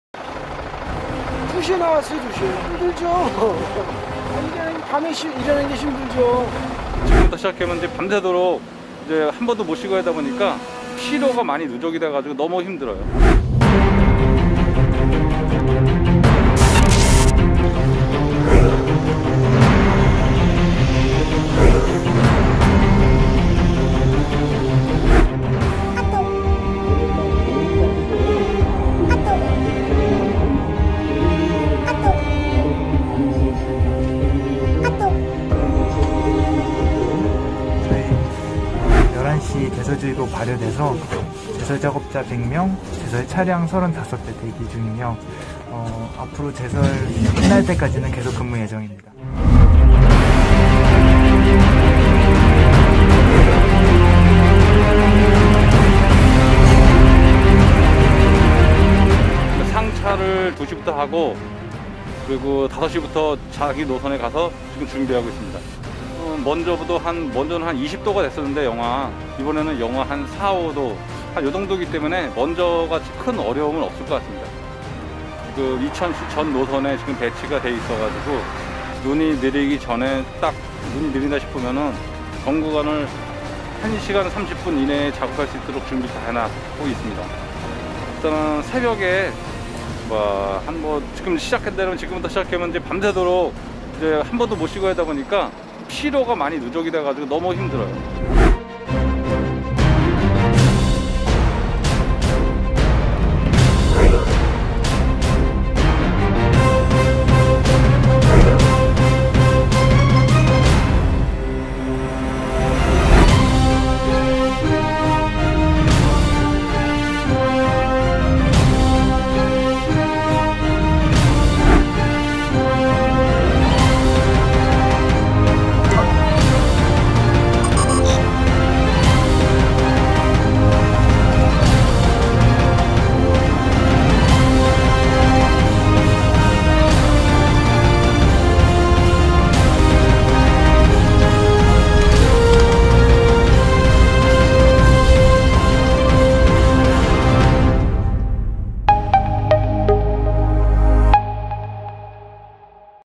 [이천은 지금] 눈이 펑펑 오는 날엔 24시간 대기모드! 이천시의 제설현장은?
[영상 이천시청 제공]

지난 1월 6일, 17일을 기억하시나요?
대설로 인해 길도 꽁꽁 사람도 꽁꽁! 얼어붙은 그 날의 끔찍한 기억들..

이천시 역시 24시간 대기하며
제설작업자 100명, 제설차 35대가 투입하여 제설에 최선을 다했습니다.

눈은 이쁘지만
우리 모두의 안전을 위해 열심히 치워야겠죠!

시민 여러분들의 안전한 출퇴근 길을 위해
앞으로의 폭설에 대비하며 최선을 다하겠습니다.